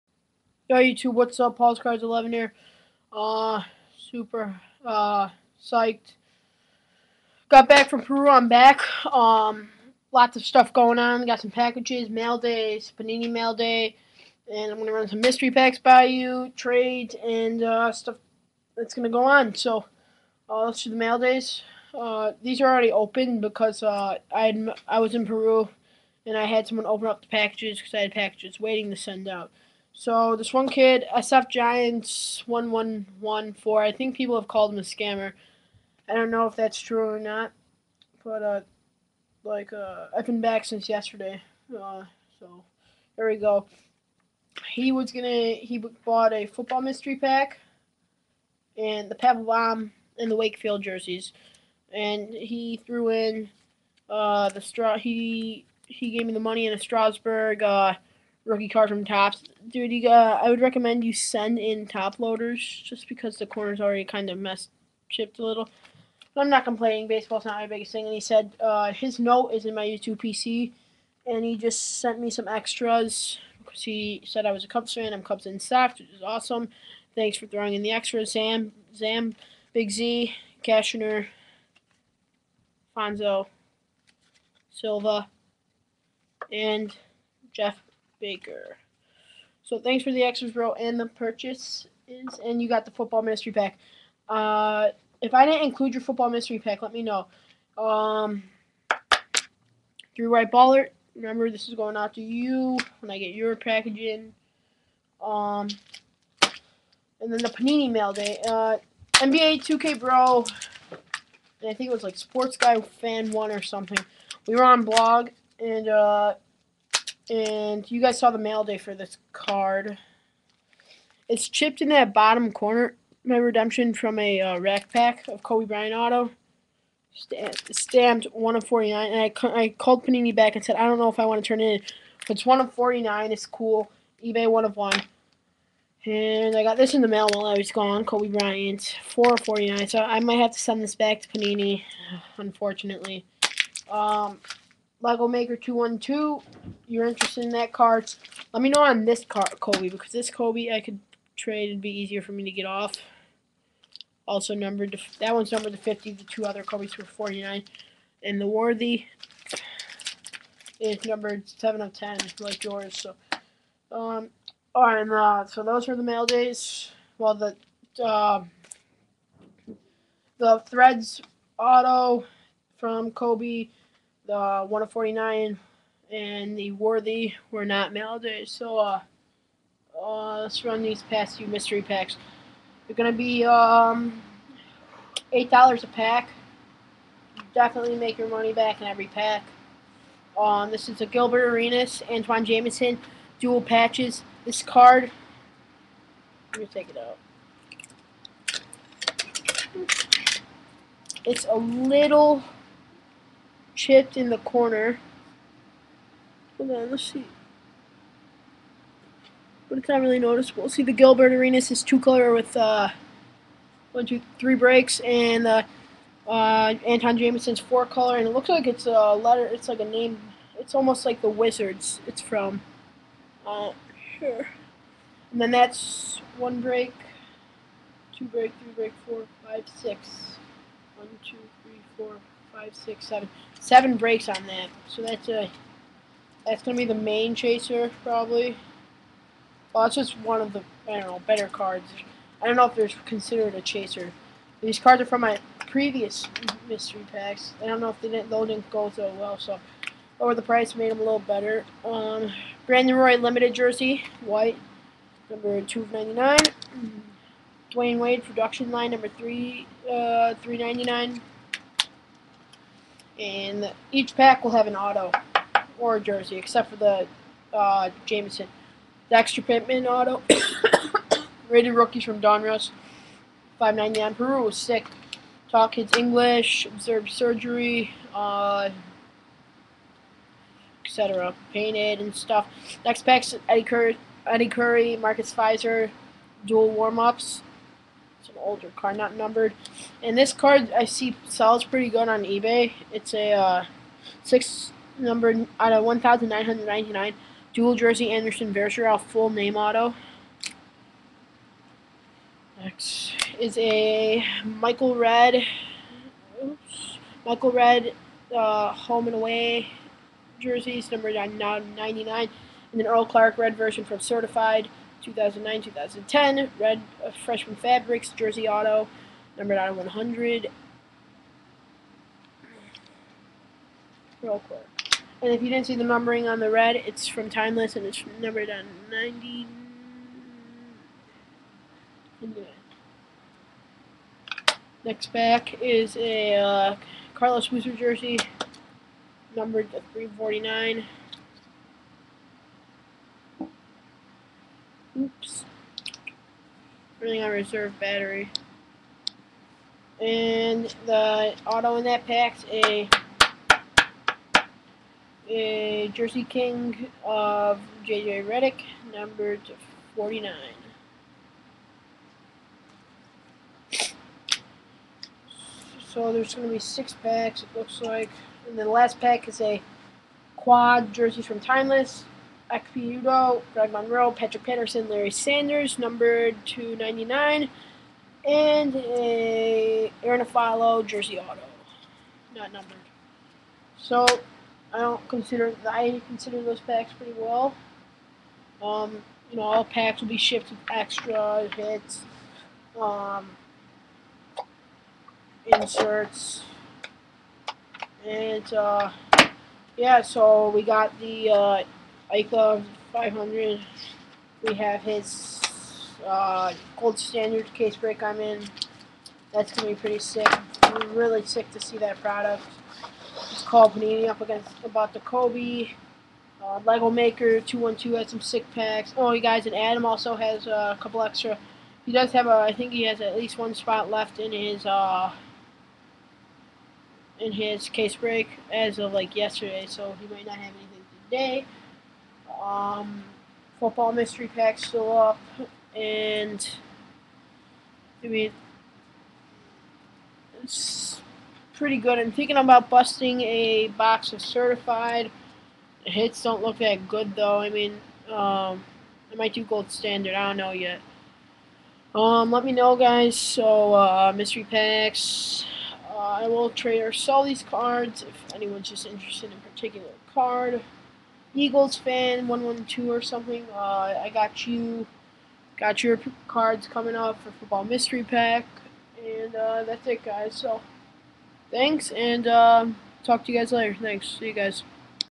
*IM BACK* 2 PACKAGE MAILDAY!PANINI AND SFGIANTS1114! MYSTERY PACKS! AND SOME NEWS!
SO LMK ON THE MYSTERY PACKS...
1.
2.
3.
4.
5.
6.
AND HAVE A GREAT WEEK EVERRYBODY COMMENT RATE AND SUB!